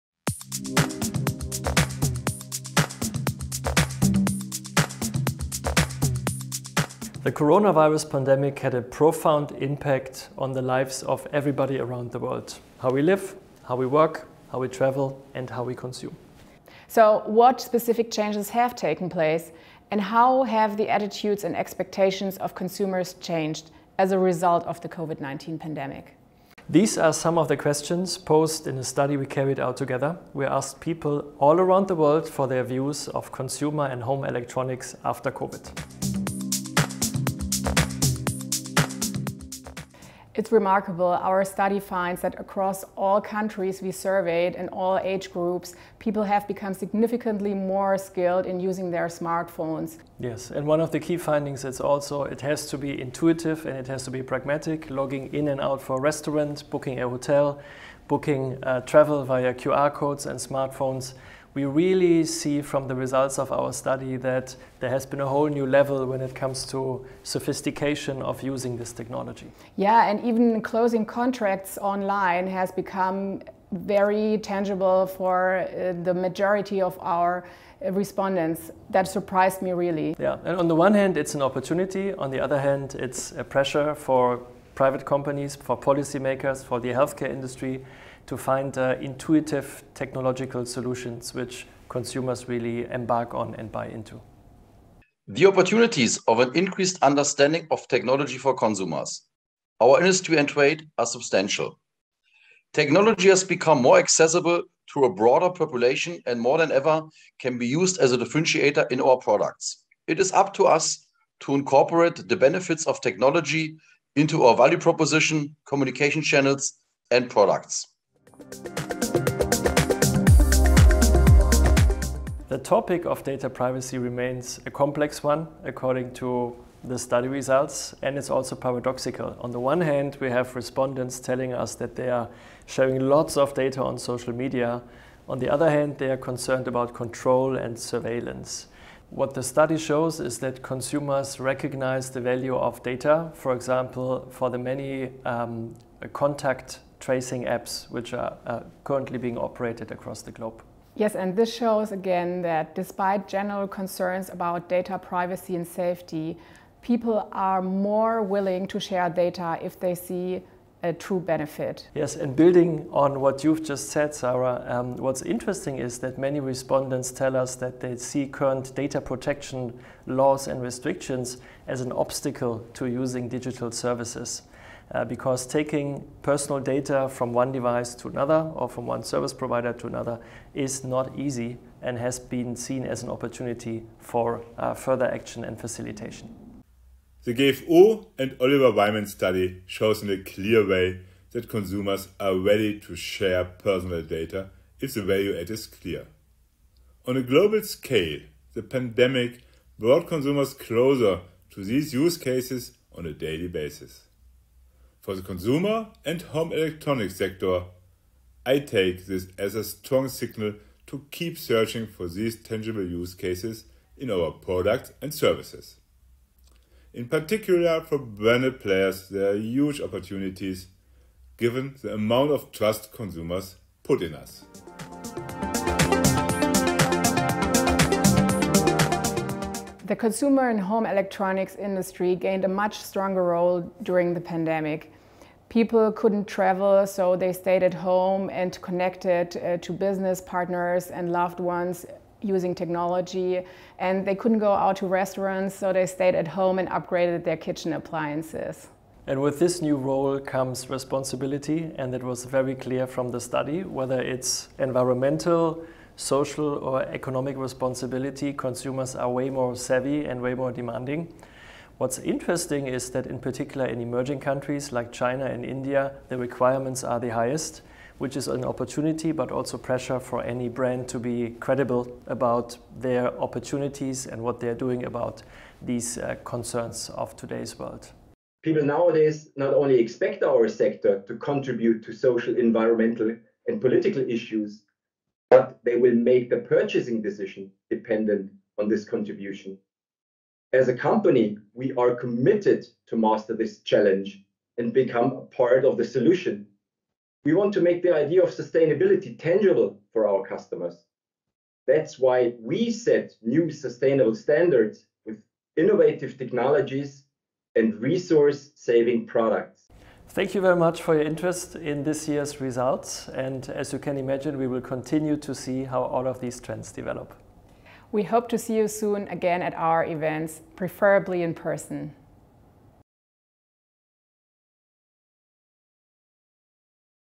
Consumer and home electronics after COVID with Oliver Wyman and gfu association
During the coronavirus pandemic, consumers have significantly improved their understanding of technology. Not only are smart phones and video conferencing systems being used with increasing confidence, but consumer attitudes to data use have also shifted along the spectrum from skepticism toward openness.

More consumers are prepared to allow more data to be stored, as long as they can see a concrete benefit in doing so. For this reason, it’s important for manufacturers to cultivate a precise understanding of customer wishes and – where possible – to generate the appropriate digital value added.

These were the findings of a global consumer survey carried out in August 2021 by strategy consultants Oliver Wyman and gfu Consumer & Home Electronics GmbH, organizer of the IFA in Berlin. For the study, consumers in China, France, Germany, India, Russia, and the US were surveyed.